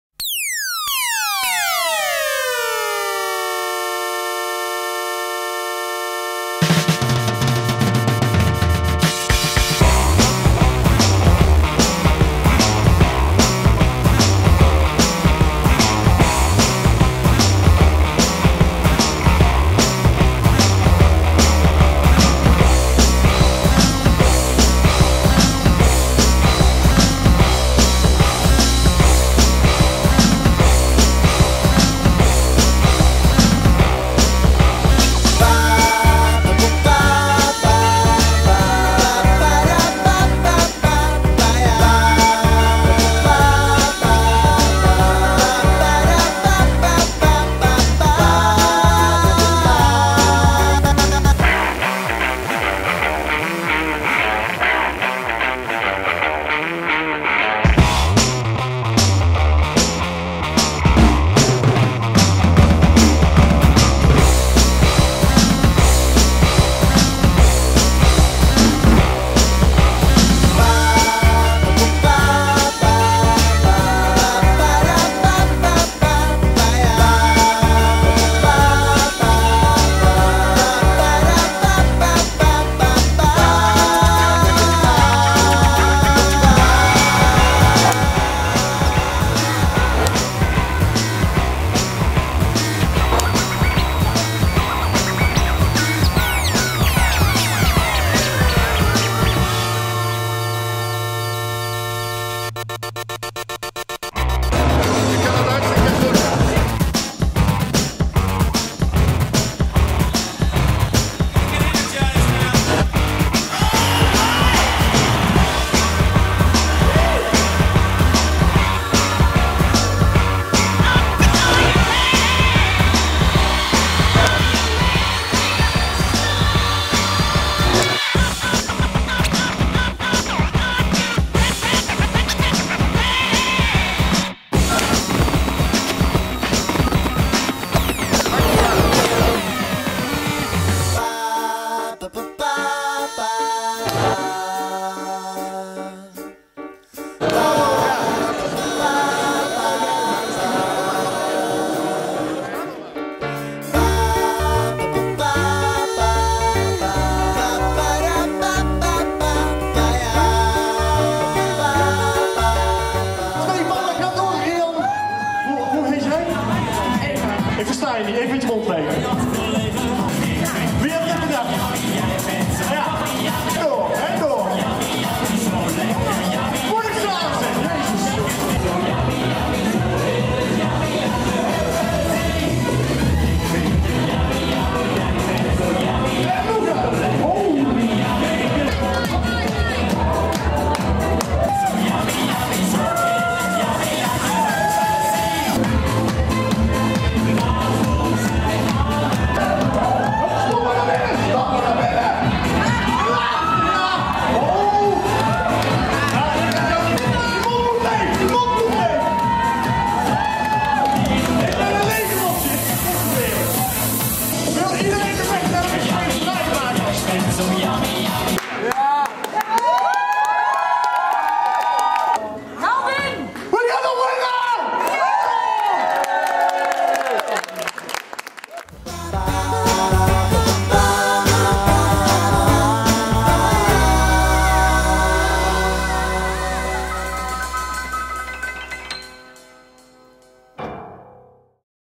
Club Roll With it meets Woof
Club Roll With it at radical theatre festival Woof, 9th of january 2009.